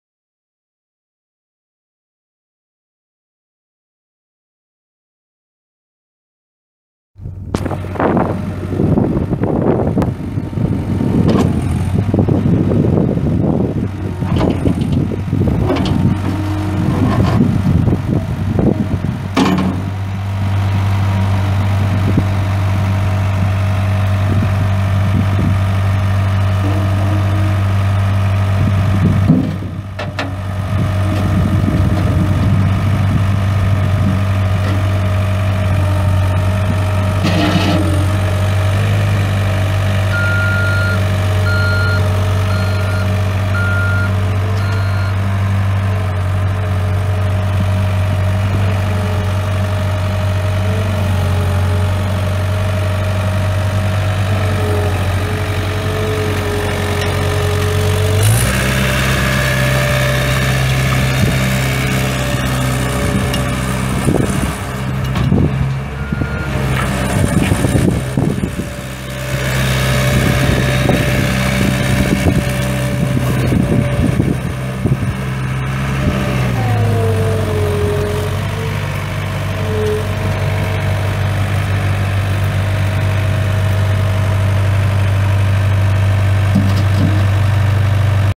2004 Caterpillar 226 skid steer for sale | no-reserve Internet auction August 31, 2017
2004 Caterpillar 226 skid steer
6,022 hours on meter
Caterpillar four cylinder diesel engine
Hydrostatic transmission
Open station/canopy
Joystick controls
Front and rear work lights
Backup alarm
Auxiliary hydraulics
10-16.5 NHS tires
Serial CATD0226C5FZ10910
Unit# 106330